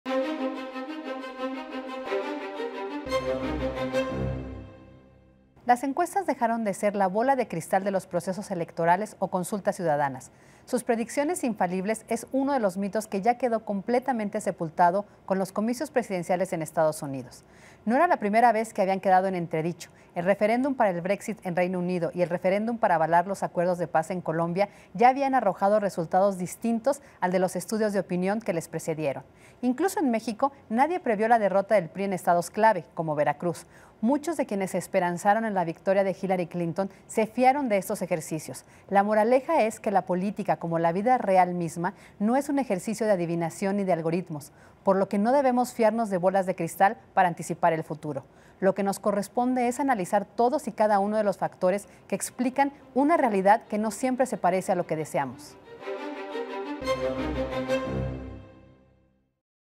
La derrota de las encuestas
Las encuestas dejaron de ser la bola de cristal de los procesos electorales o consultas ciudadanas.